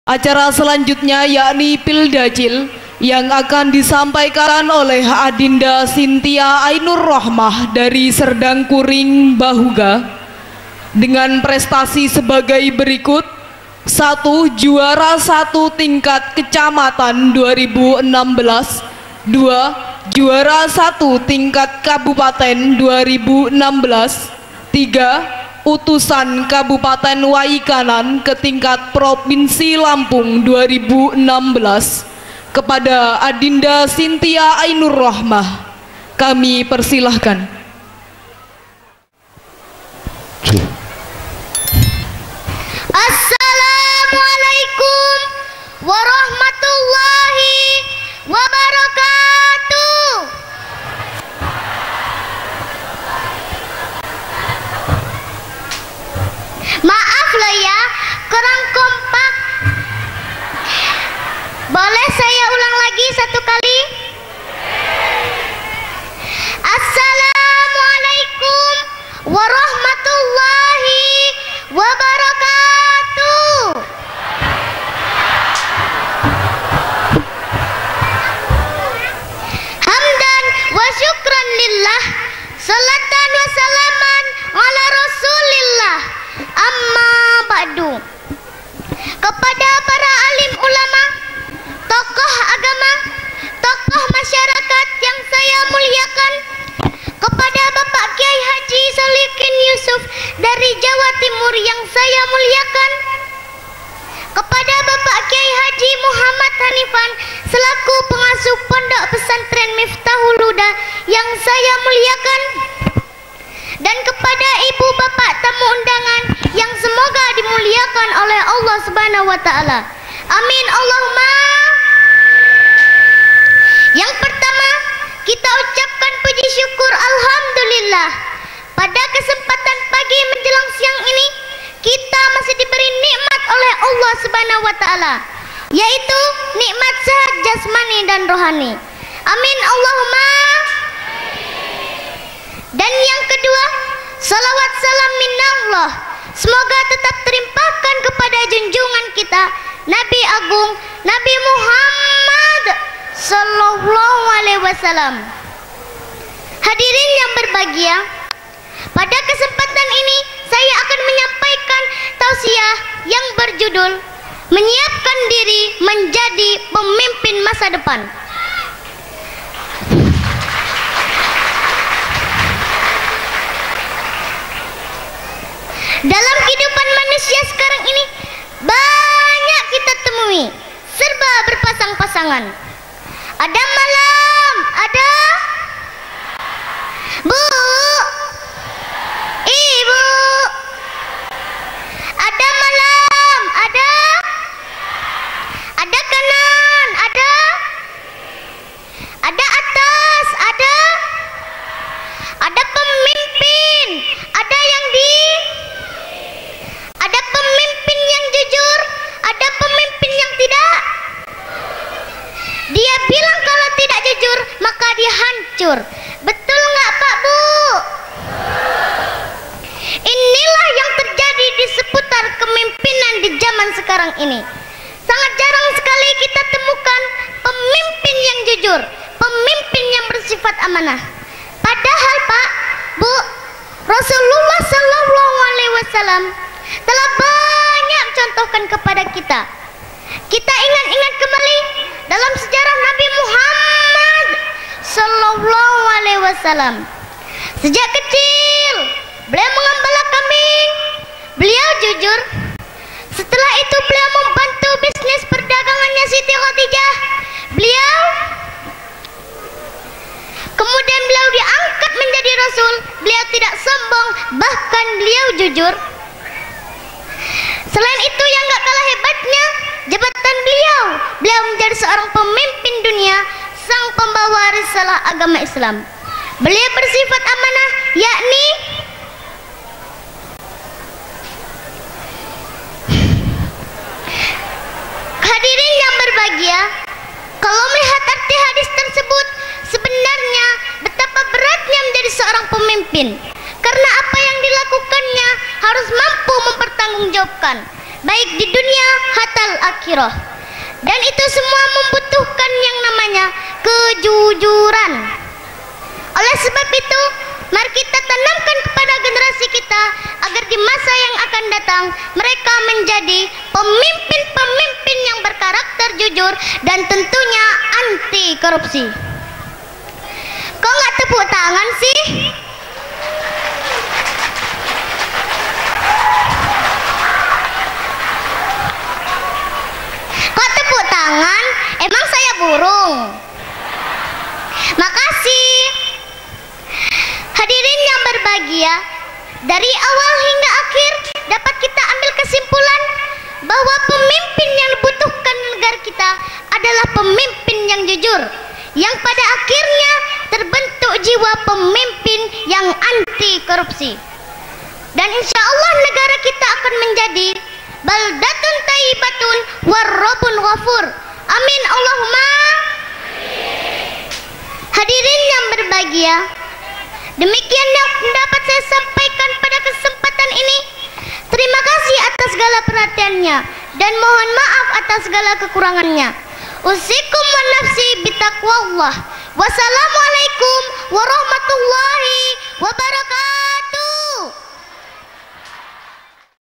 Viral!!! Dai cilik membuat ribuan pengunjung terpesona!! Menyiapkan diri menjadi pemimpin masa depan
Viral!!! Dai cilik Chintya Ainur Rohmah membuat ribuan pengunjung terpesona!!! tema menyiapkan diri menjadi pemimpin masa depan. dalam rangka haflah akhirussanah th. 2018 PP Miftakhul Huda desa Bumiharjo kecamatan Buay Bahuga kabupaten way kanan